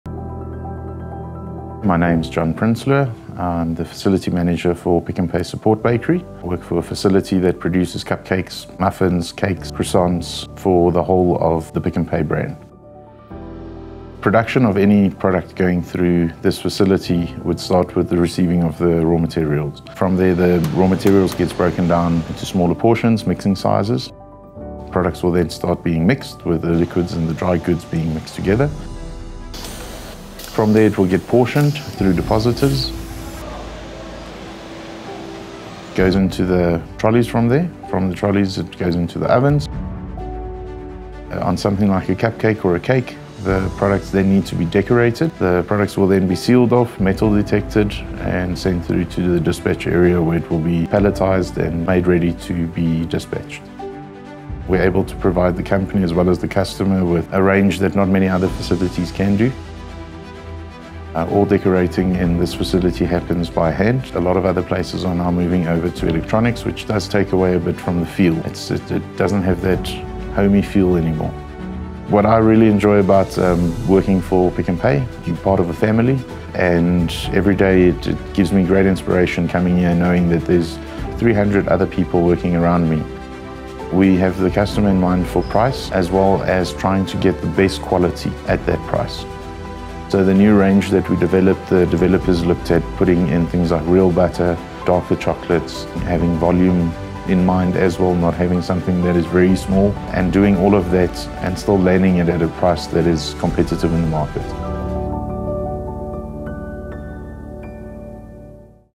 Pick n Pay Trusted Suppliers: Bakehouse
The flawless operational and logistical symphony required to craft innovative baked goods for Pick n Pay stores across the country is immense. Here, our bakehouse faculty manager John Prinsloo shares what it takes to manage this kind of operation every day.

John has been working for Pick n Pay from the start of his career and has worked his way up to the faculty manager through his commitment to his craft. He leads a passionate team of individuals who know what it takes to delight our customers daily.